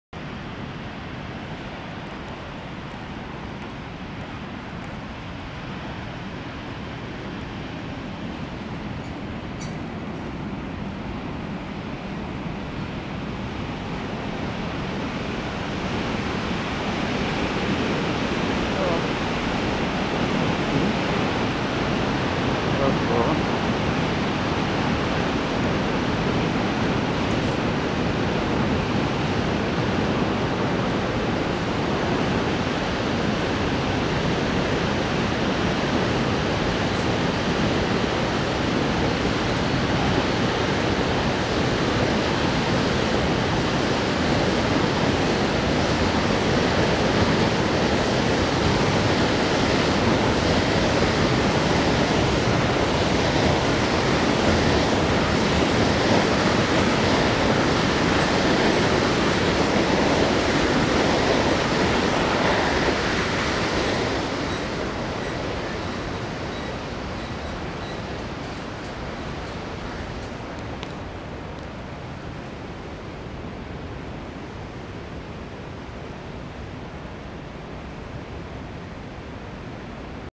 Feet on Elk Falls Suspension Bridge
It's a walk across the Elk Falls Suspension Bridge ... without music.